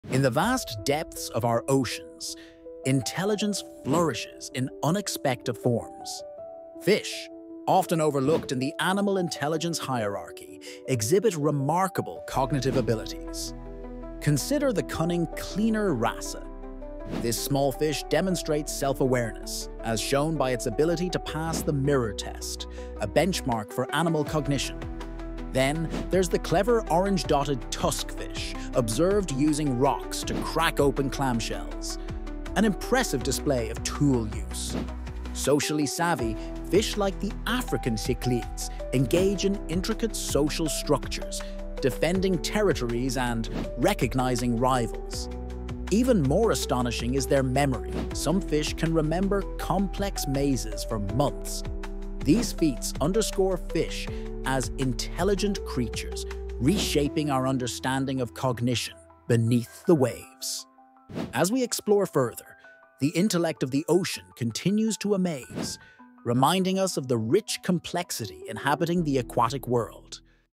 Unveiling Hidden Genius: The Surprising Intelligence of Fish in the Deep Blue
You can create faceless videos like this too using Syllaby!

Dive into the captivating world beneath the waves, where intelligence thrives in the most unexpected places. Our new video uncovers the surprising cognitive abilities of fish, creatures often underestimated in the animal kingdom. Meet the cunning cleaner wrasse, a small fish exhibiting self-awareness by passing the mirror test, and the resourceful orange-dotted tuskfish, known for using rocks to crack open clamshells—a remarkable feat of tool use. Explore the intricate social dynamics of African cichlids and be amazed by the impressive memory of certain fish, capable of recalling complex mazes for months. These discoveries challenge our perception and reveal the profound intellect that resides within the ocean's depths. Join us on this eye-opening journey to appreciate the rich complexity and intelligence that define our aquatic neighbors.